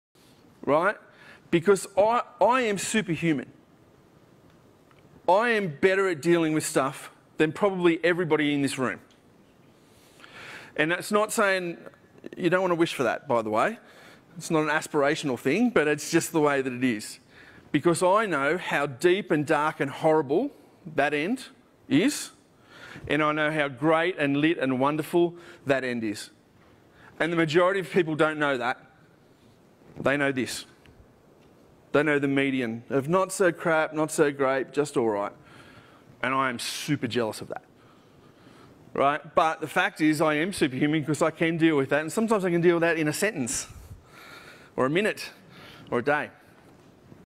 [Throwback November 2017] I am superhuman.
I am superhuman. I choose to reframe and re-tell my story everyday. I can see my character defects and flaws as just that, or I can see them as my superpowers, the things that make me superhuman. One helps me, one does not.